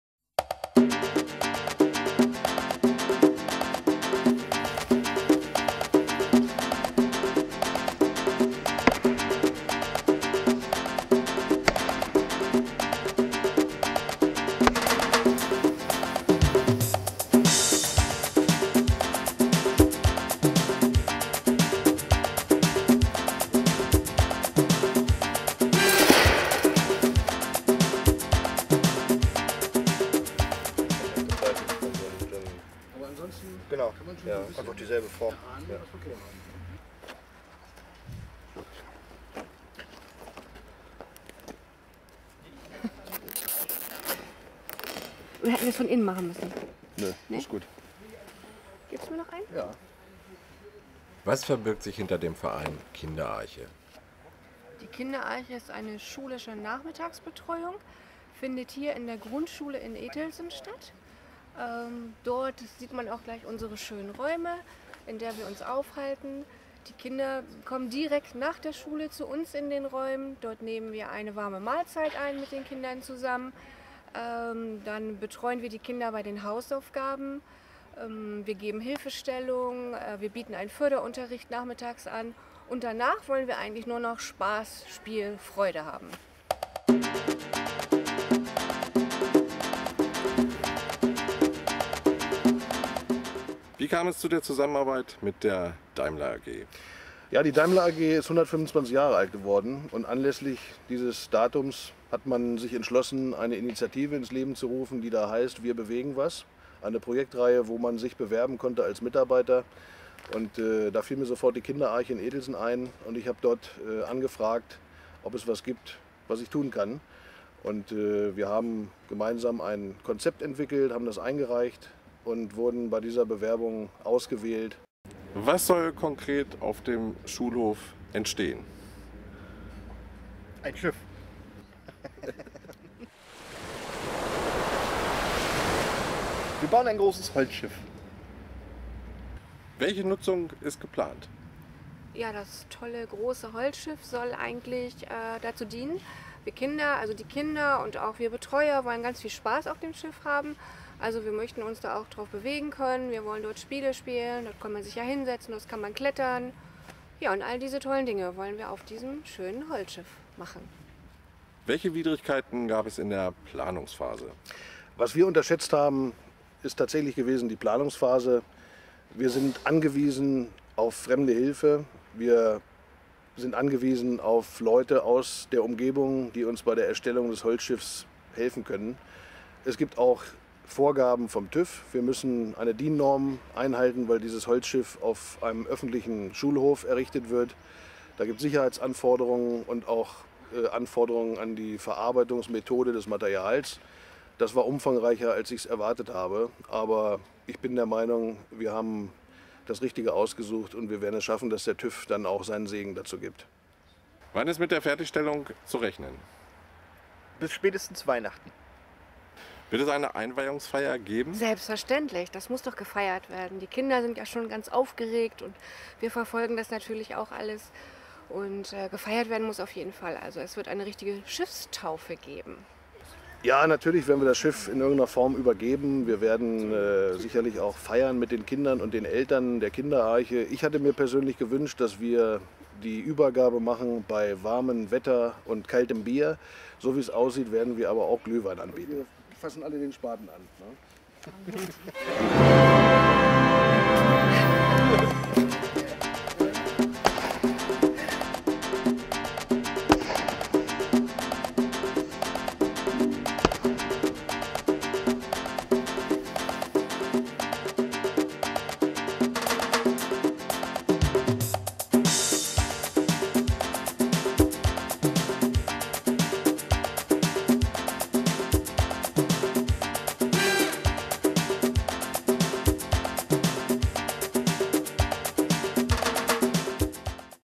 Kinderarche Etelsen - Wir bauen ein Schiff aus Holz
Wir haben großes vor. Ein tolles Schiff für unsere Kleinen. Wir sind für weitere Mitarbeit von Eltern oder Firmen dankbar.